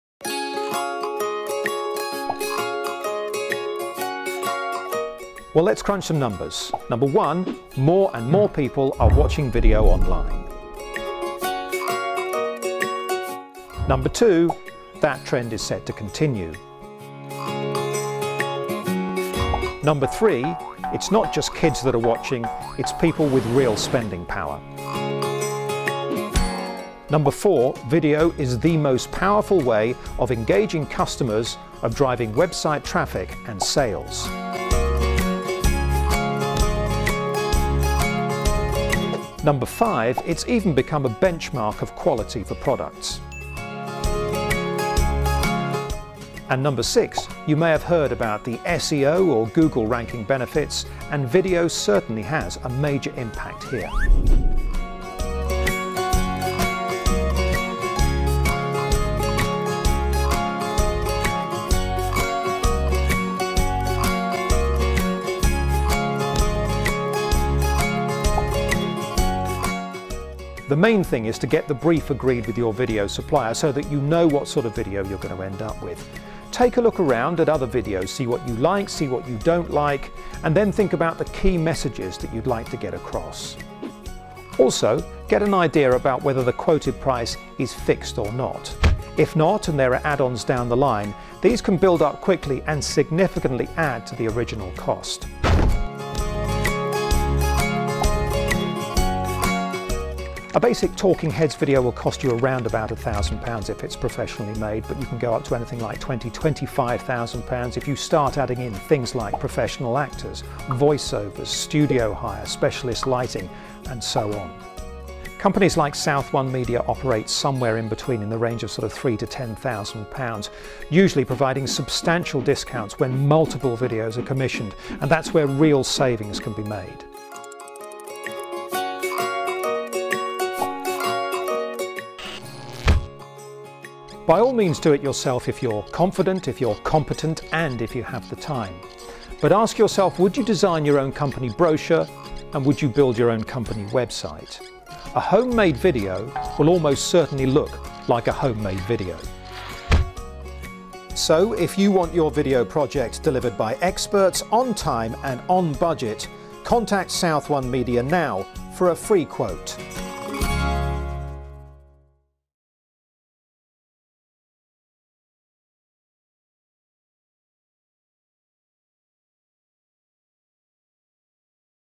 Your Web Video Questions. Answered by SouthOne Media - Hampshire Corporate Film Production.
If you're thinking of having a video made for your website, South One Media have produced a short video to answer some of your questions.
Please get in touch if there's anything else you need to know.